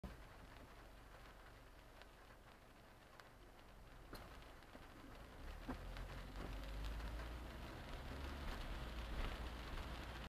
Red light runner in the rain
Blatant red light runner.